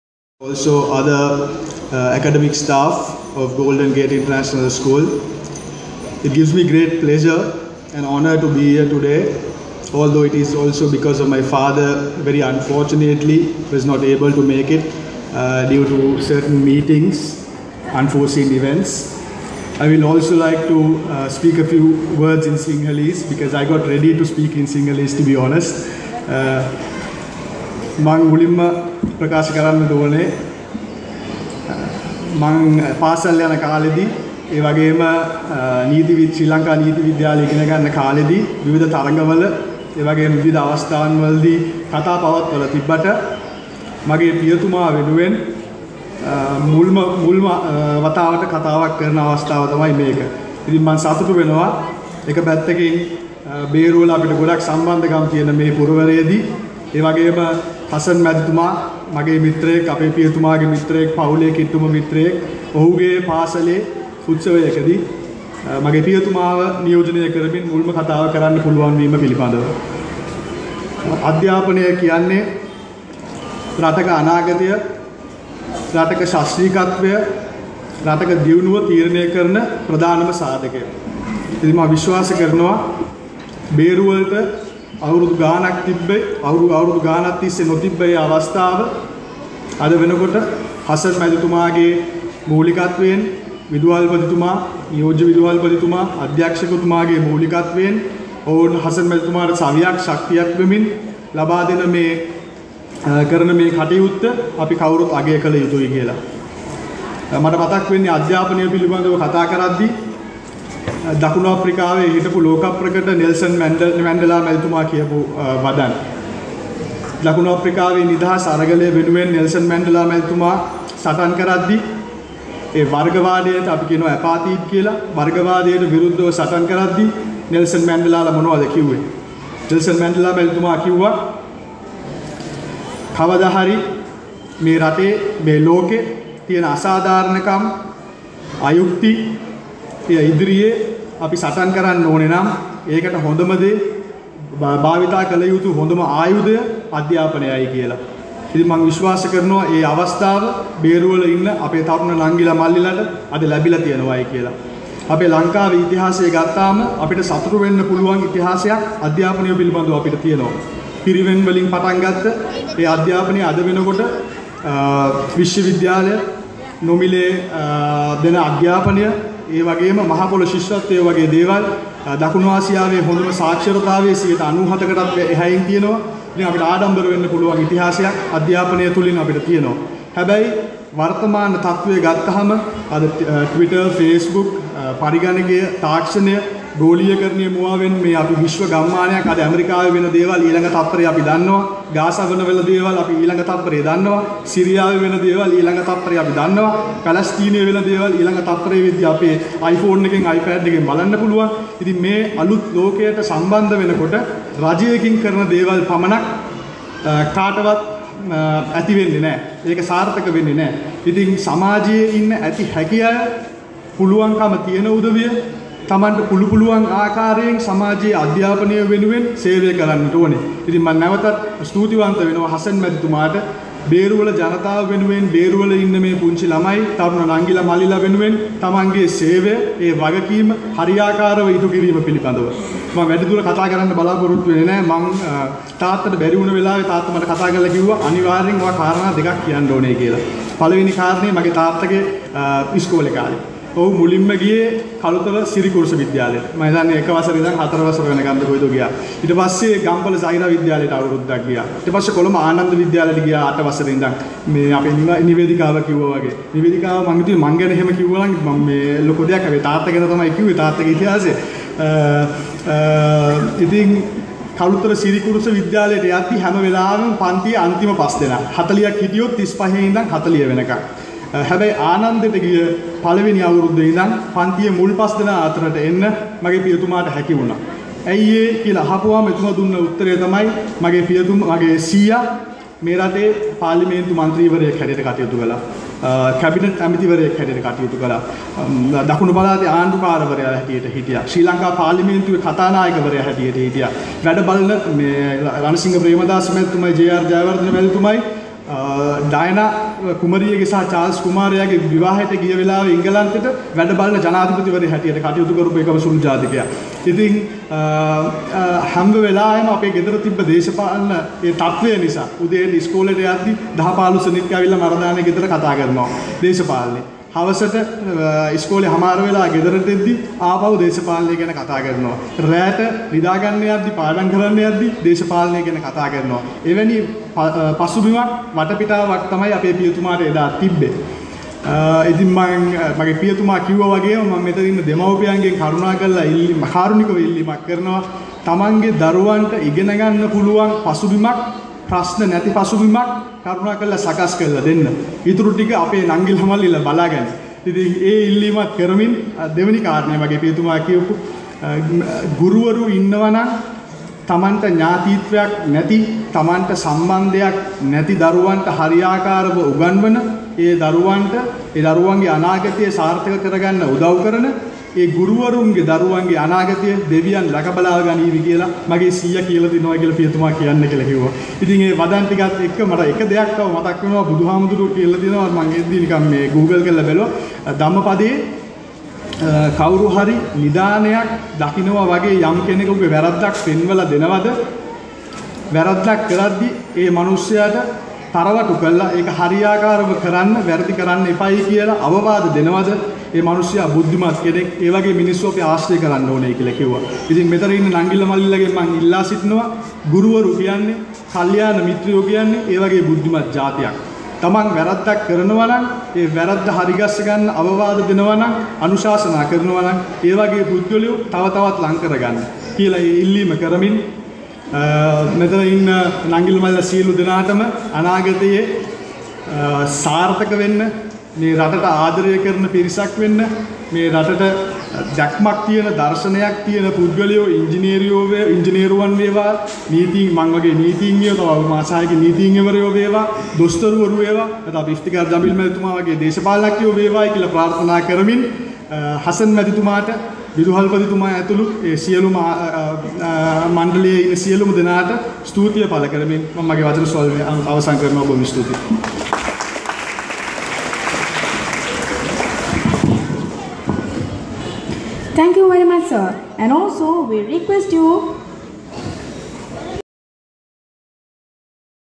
Adhil Bakeer Markar speaking at the Annual Prize Giving of Golden Gate International School
Speaking at the Annual Prize Giving of Golden Gate International School, Beruwala held on the 23rd of November, 2014.